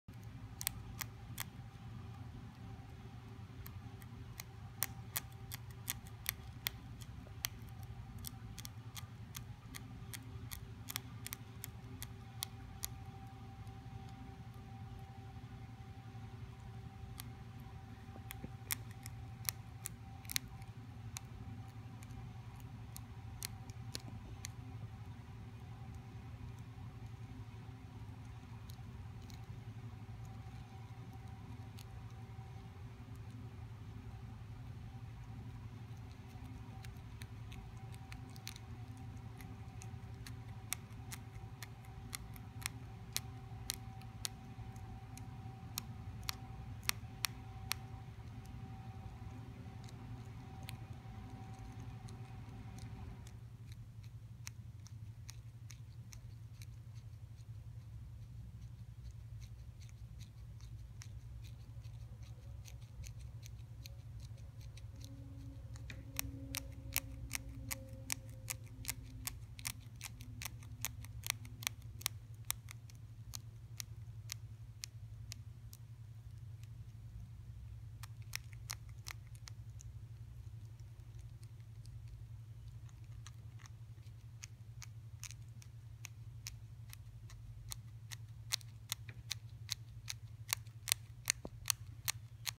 Luna Moth chewing the pupa
Video of one of our three Luna moths chewing her way out of the pupa. This was part of the Science projects for our Living Science workshop for Elementary and Middle.